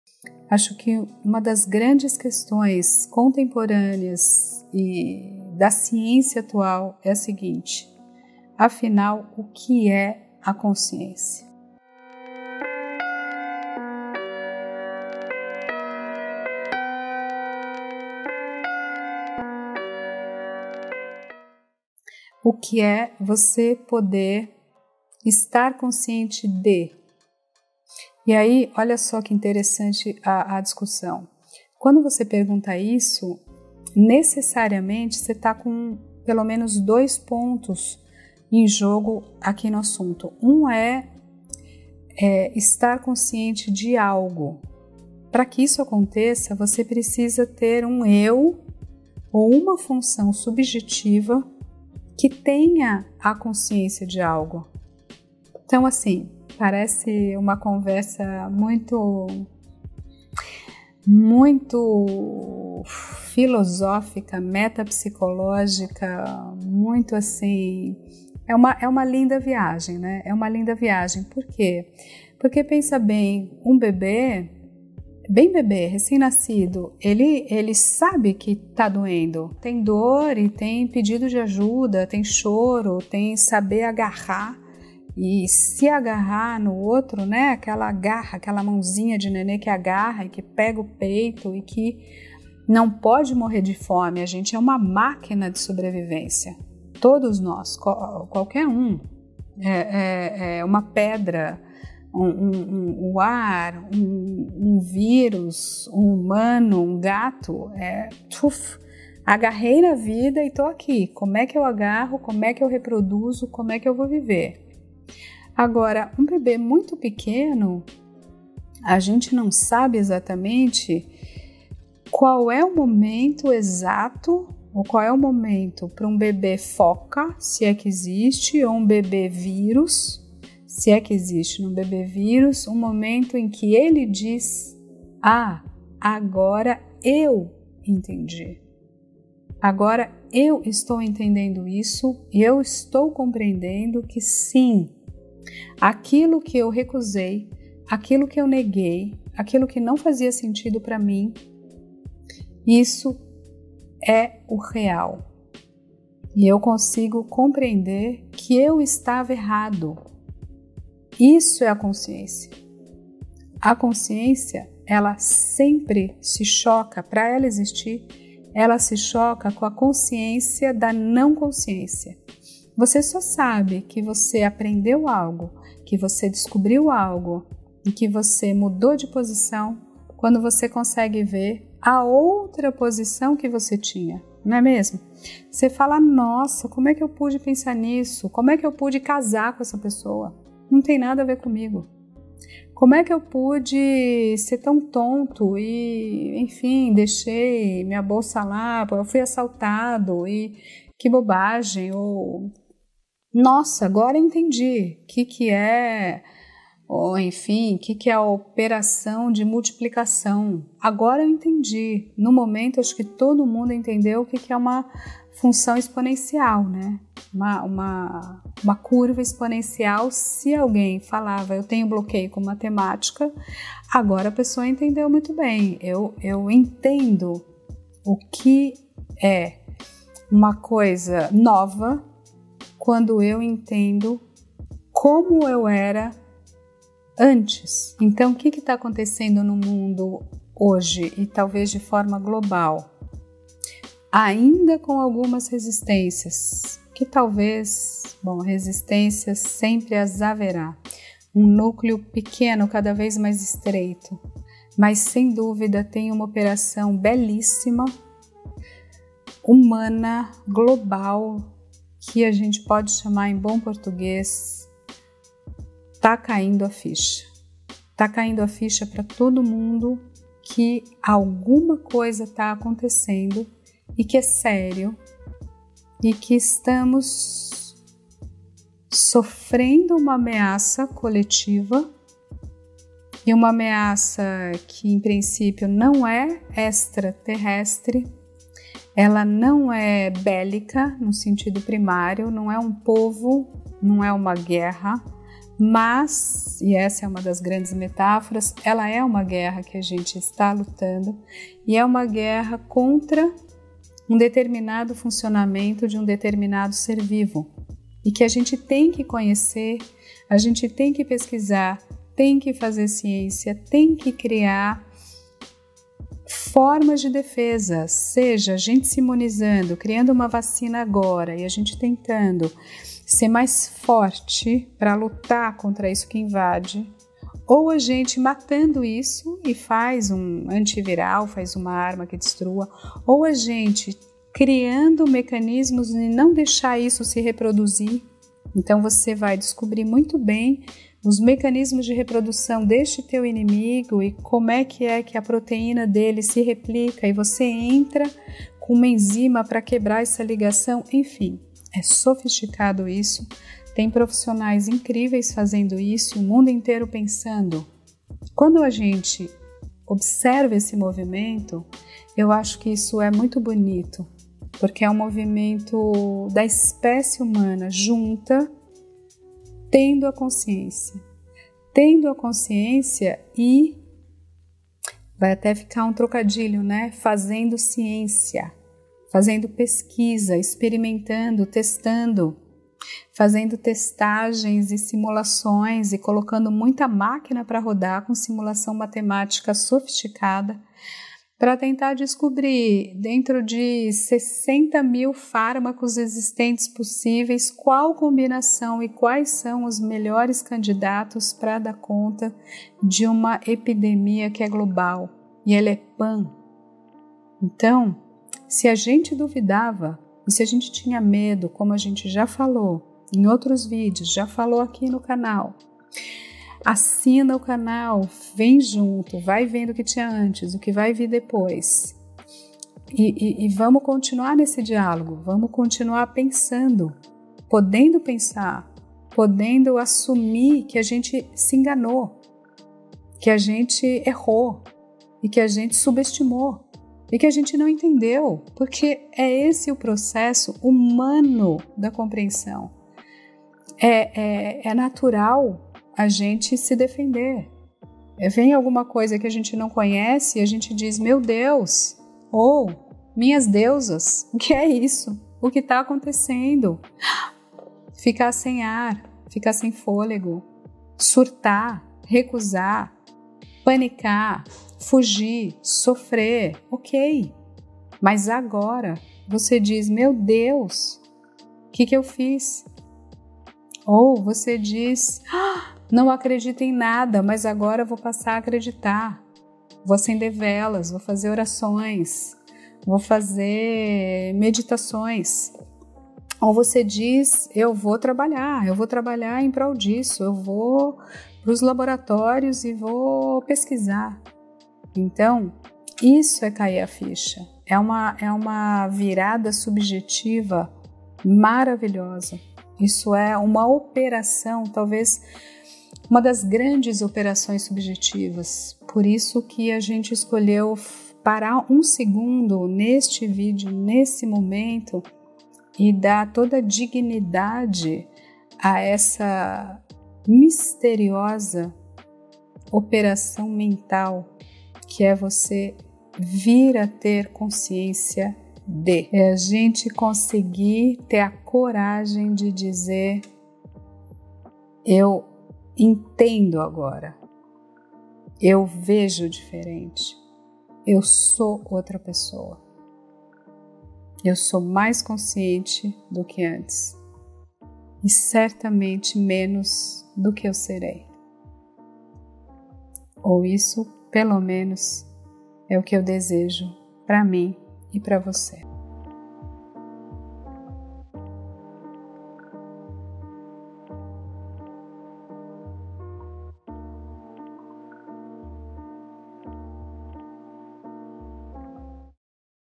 MARIA HOMEM: AFINAL, O QUE É A CONSCIÊNCIA?
Uma das grandes questões contemporâneas é a seguinte: afinal, o que é a consciência?

Livro 'Coisa de menina?' (Papirus, 2019)